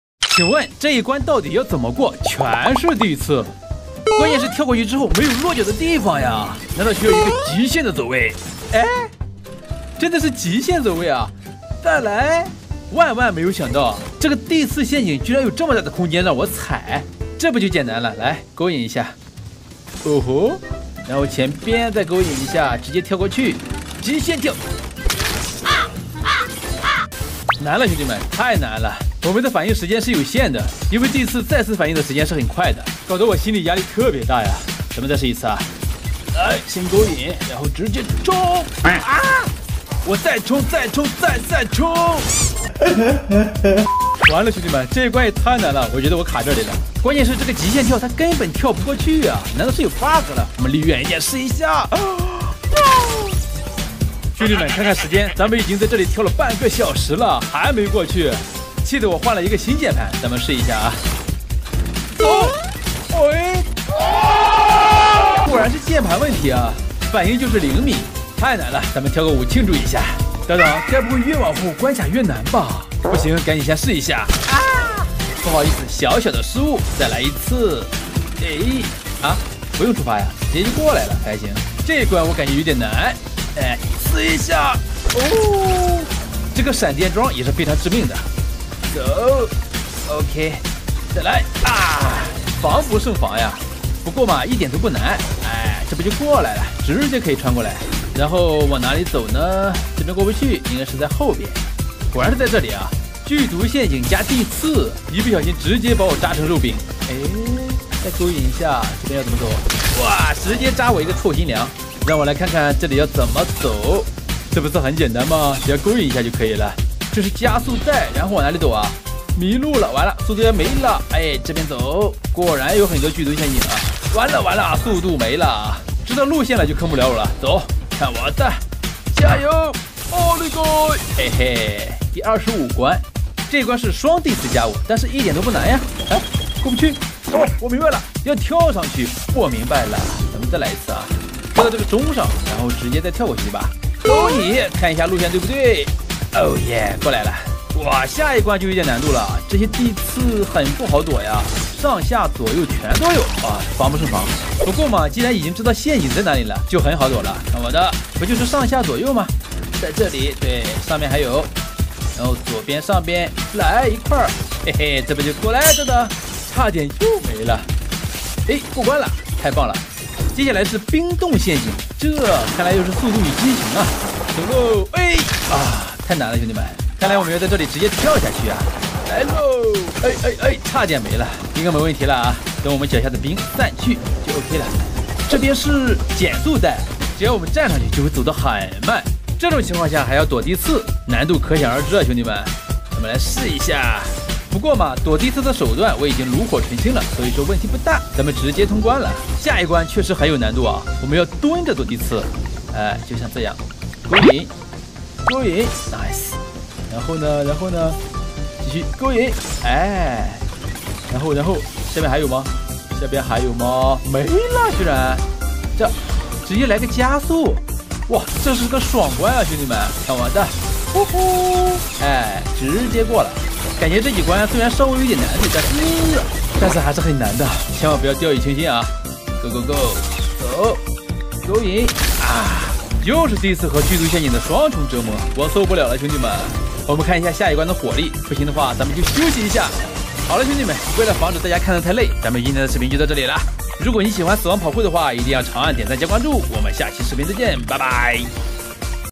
君扬兄：只有1滴血挑战超难死亡跑酷！第一关跑了半小时！
❤️ 這裡充滿了很多有趣的影片！
🔥 超級精彩的影片播放清單 🔥